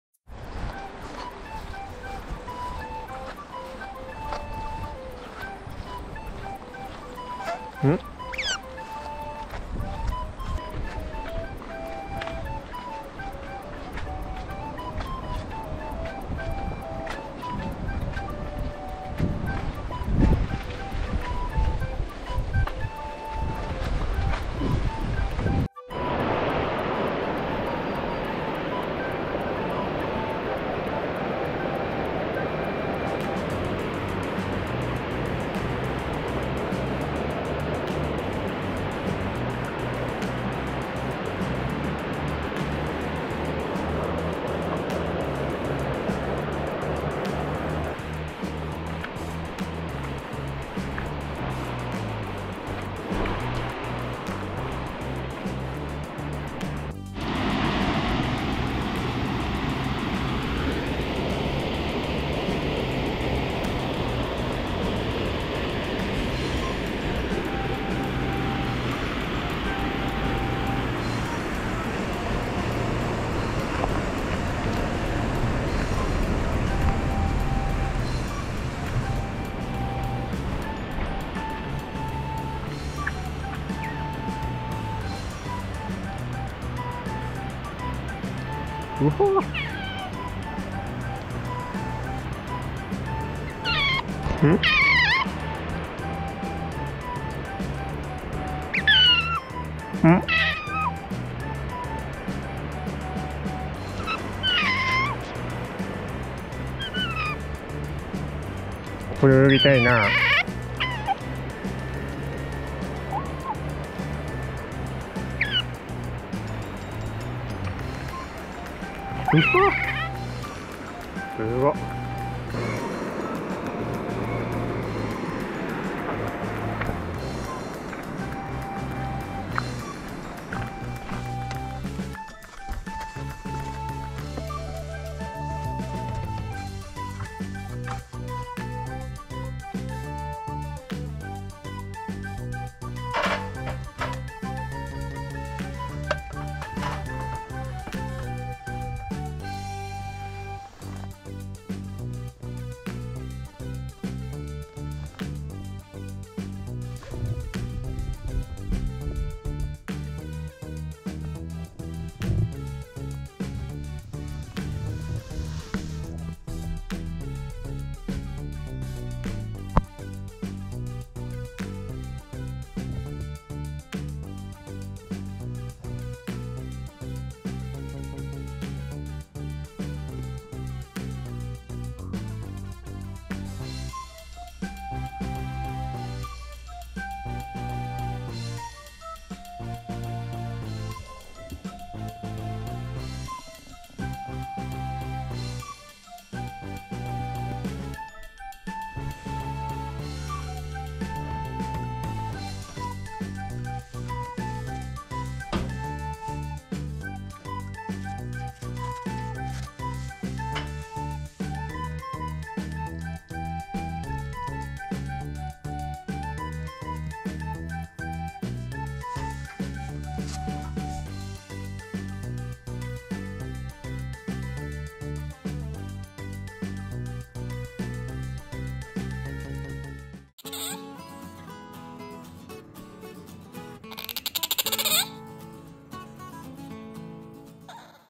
Explore Uotobikei, famous for its spectacular scenery of the valley! [Otter life day 358]
There were a number of spots where we could swim.
But we didn't want to go down to the river this time because it's quite a dangerous place to go down to the river.
We'd love to swim here someday!